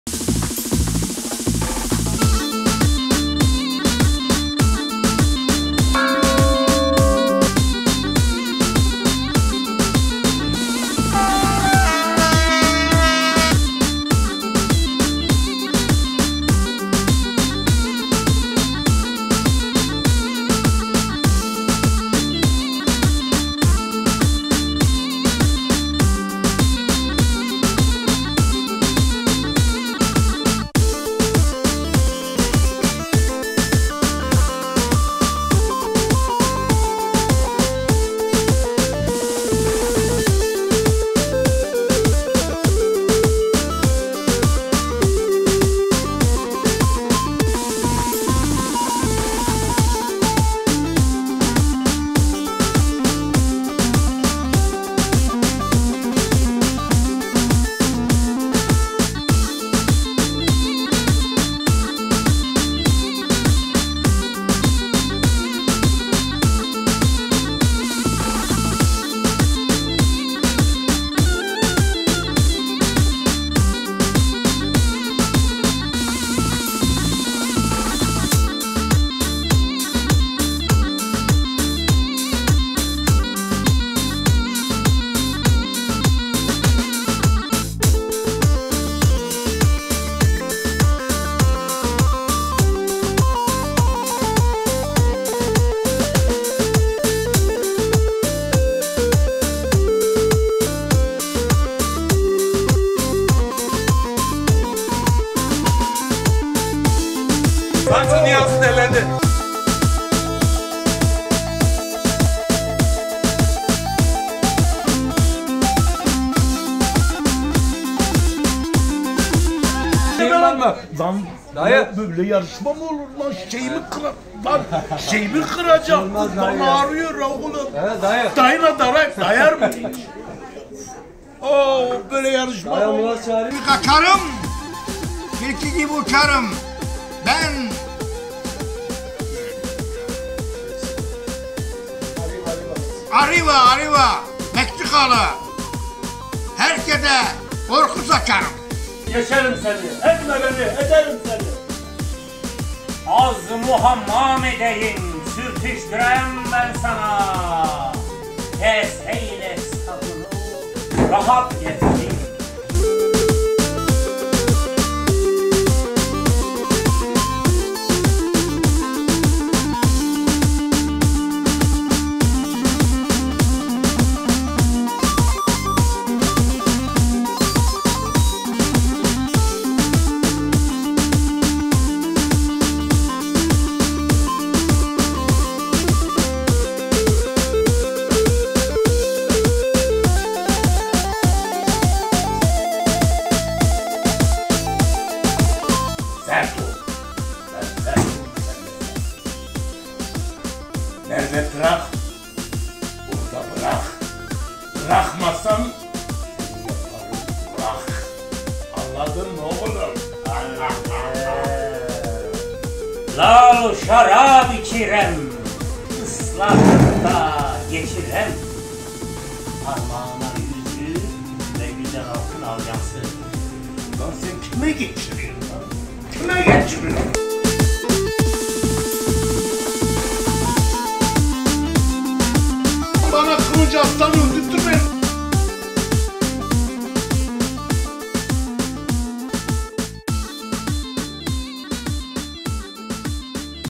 ZOR-VAY-VIR (Balon oyunu ve atışma)
Adana usulü cıncık gibi yarışma ZOR VAY VIR bu defa balon oyunu ile devam etti. Arkasından gelen atışma oyunu ile bazen sinirler gerildi. Tikli dayı ayakkabısı ile Kılıç aslanın peşine düştü:))